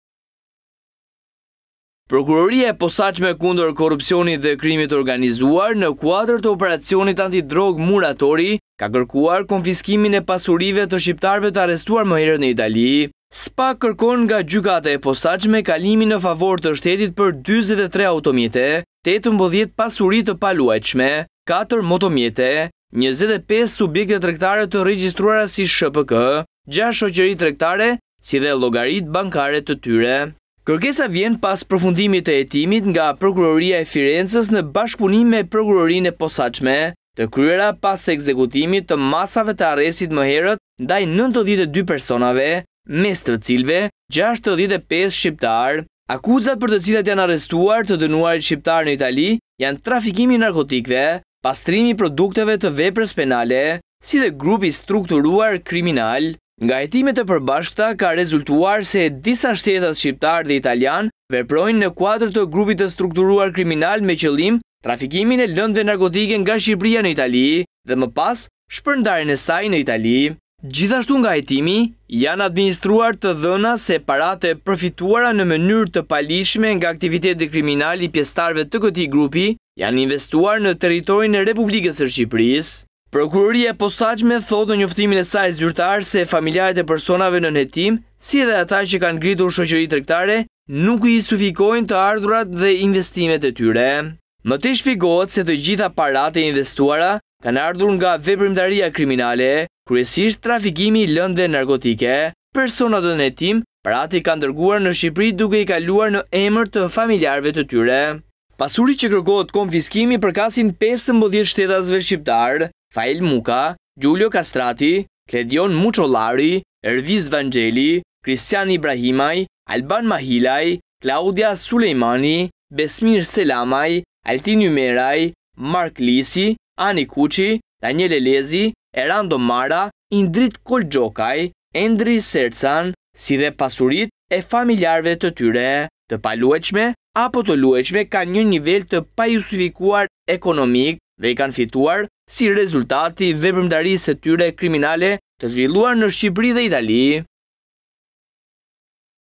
Pastruan paratë e drogës në ndërtim. SPAK kërkon konfiskim për 25 subjekte tregtare dhe 43 automjete
Prokuroria e Posaçme Antikorrupsion i kërkon gjykatës konfiskimin e pasurive të 15 personave që dyshohet se e kanë prejardhjen nga aktivitetet e paligjshme të familjarëve të tyre në Itali. Në listën e pasurive që kërkohet t'i kalojnë shtetit janë 18 pasuri të paluajtshme, 25 subjekte tregtare, 6 shoqëri tregtare dhe 43 automjete.